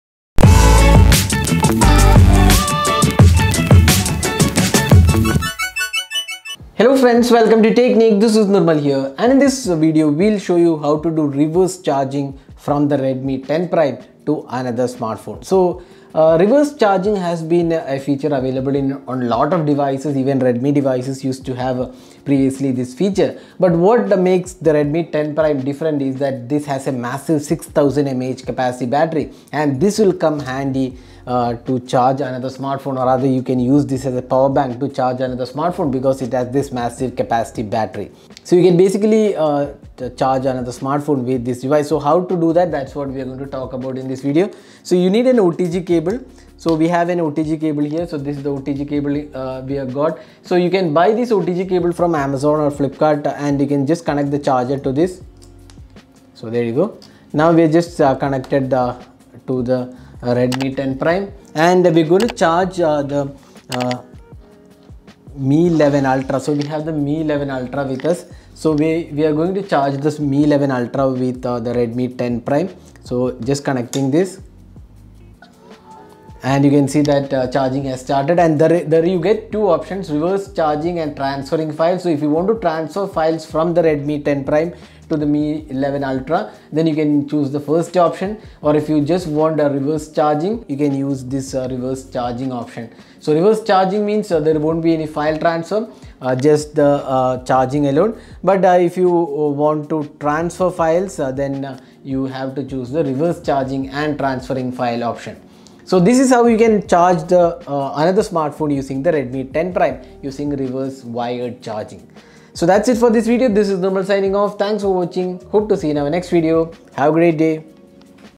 Redmi 10 Prime Reverse Wired Charging
Redmi 10 Prime offers reverse charging using the 6000mAh battery and here is how to use it.